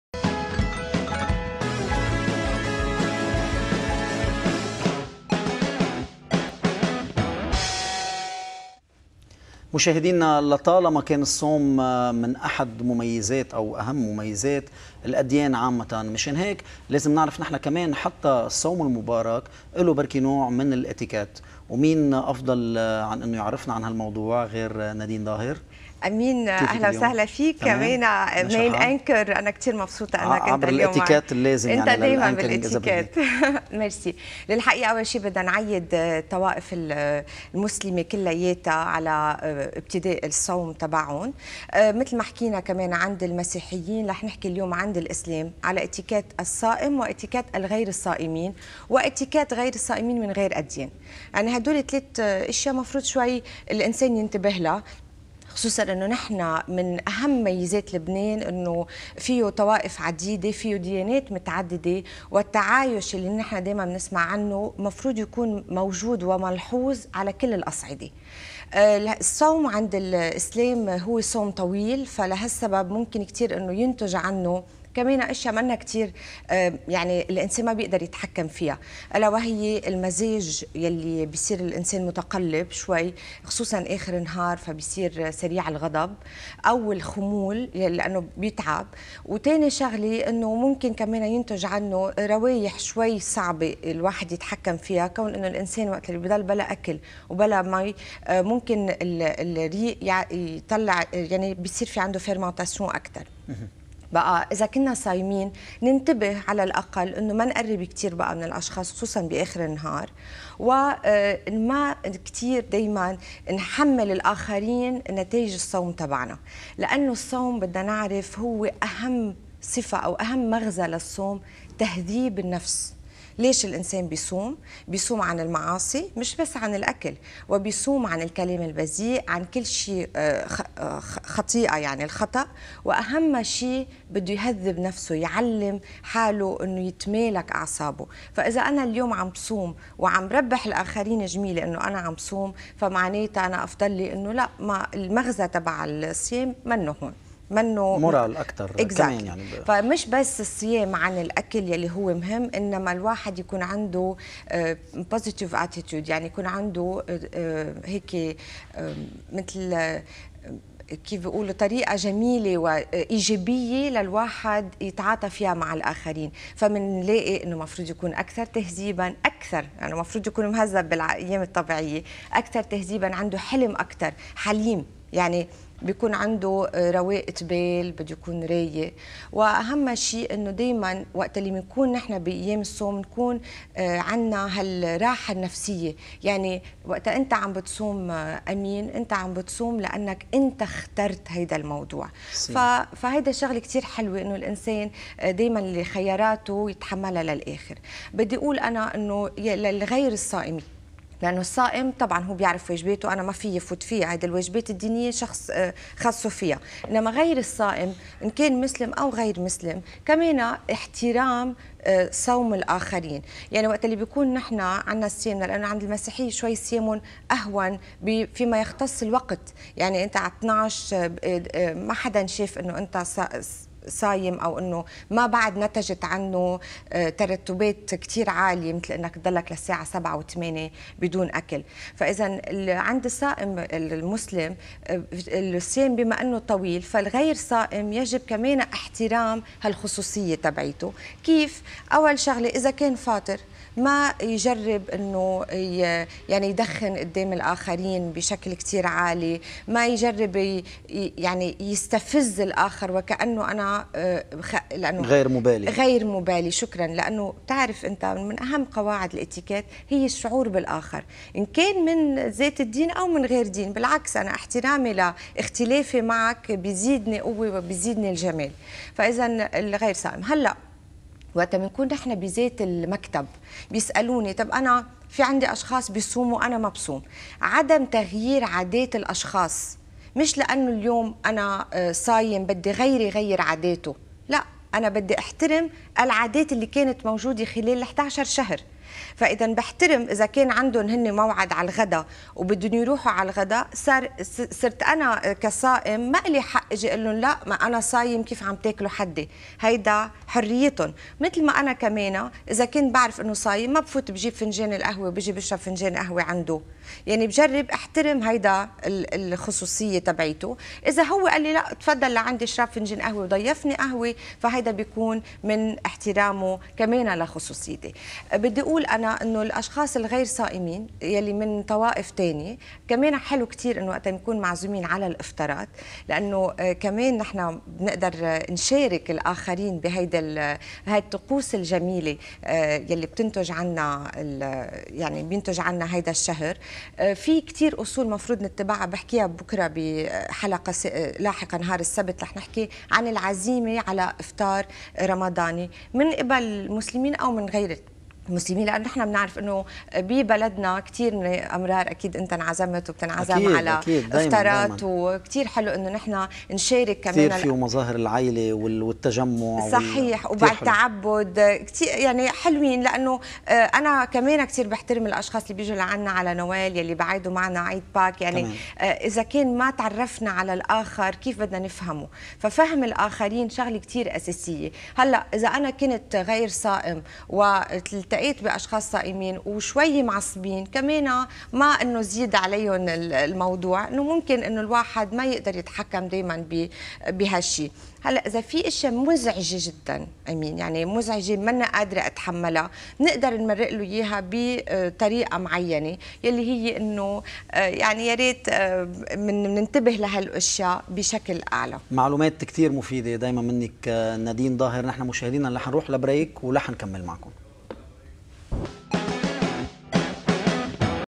Etiquette - 11/07/2013
في اتيكيت اليوم أصول الصوم خلال شهر رمضان